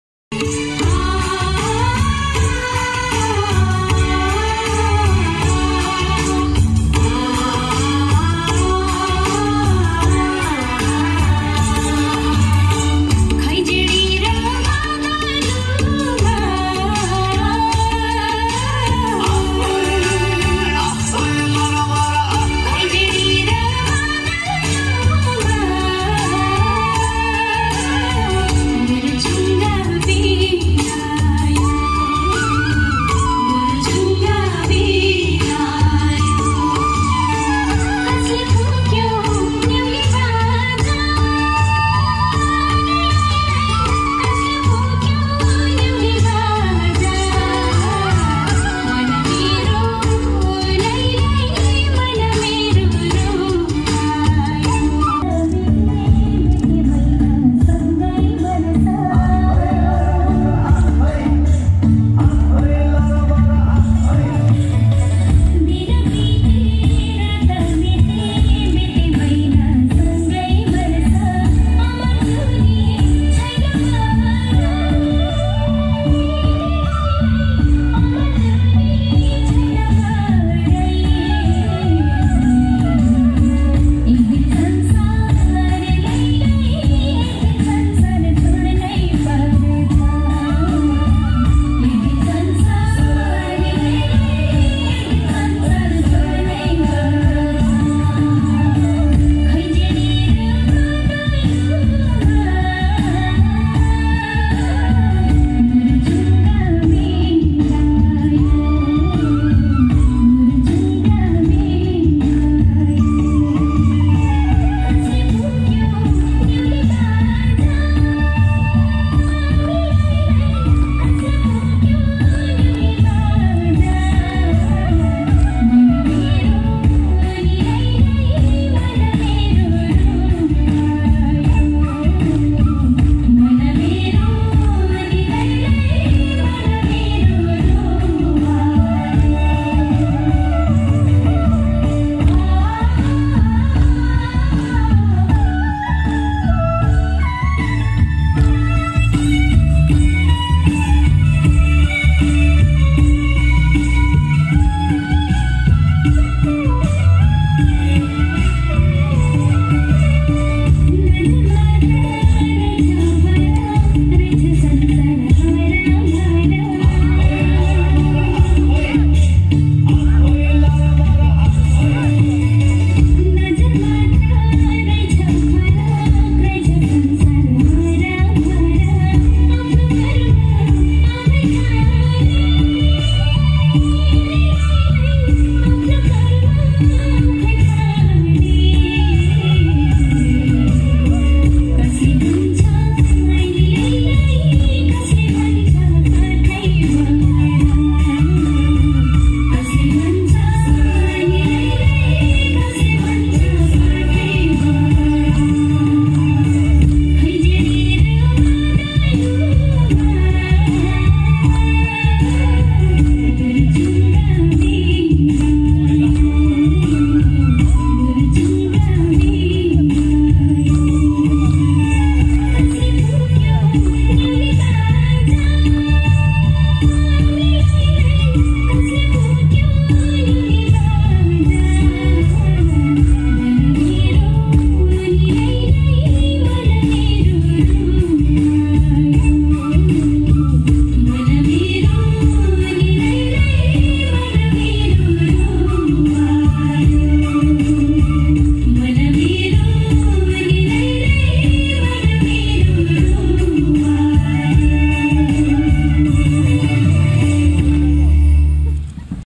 Maruni Dance | टुडिखेलमा मारूनी
गणतन्त्र दिवस २०७५ को अवसरमा मारूनी नृत्य प्रस्तुत गर्दै नेपाल प्रहरीको साँस्कृतिक टोली ! २०७५ जेठ १५